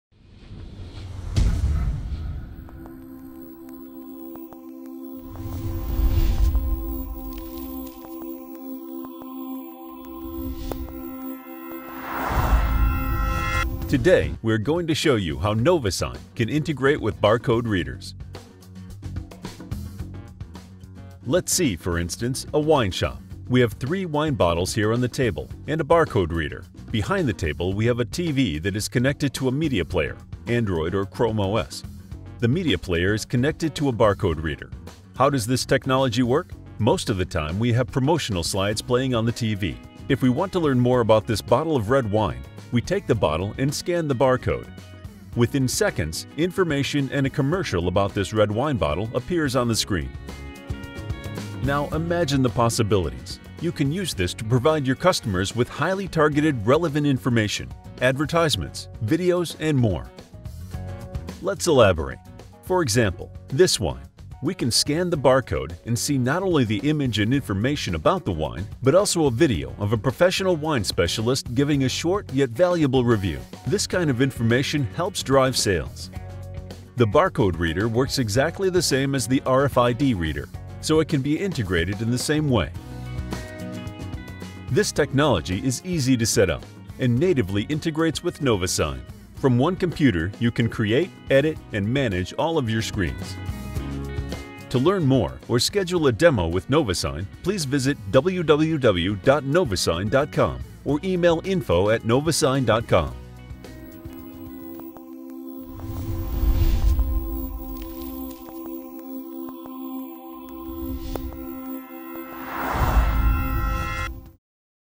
How can I use barcode readers with digital signage?
Today we are going to show you how NoviSign’s Digital Signage can integrate Digital Signage with barcode readers.
Let's see, for instance, a wine shop: we have three wine bottles here on the table and a barcode reader. Behind the table, we have a TV that is connected to a media player (Android or Chrome OS). The media player is connected to a Barcode reader.
What is barcode digital signage?
Most of the time, we have promotional slides playing on the TV. If we want to learn more about this bottle of red wine,  we take the bottle and scan the barcode. Within seconds, information and a commercial about this red wine bottle appears on the screen.
Now, imagine the possibilities:
You can use this to provide your customers with highly targeted relevant information, advertisements, videos and more! Now, imagine how you can use this technology for your retail digital signage?
Let's elaborate:
For example, this wine: we can scan the barcode and see not only the image and information about the wine, but also a video of a professional wine specialist giving a short yet valuable review. This kind of information helps driving sales!
The barcode reader works exactly the same as the RFID reader, so it can be integrated in the same way.
This technology is easy to setup and natively integrates with NoviSign.